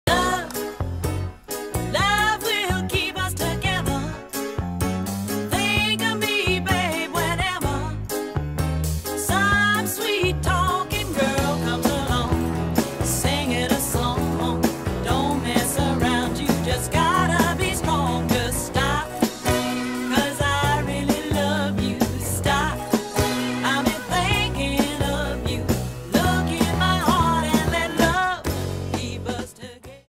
Captain and Tennille Miniature Pinschers for adoption!!
These two MinPins are for adoption in Southern California with IMPS minpinrescue.org  via YouTube Capture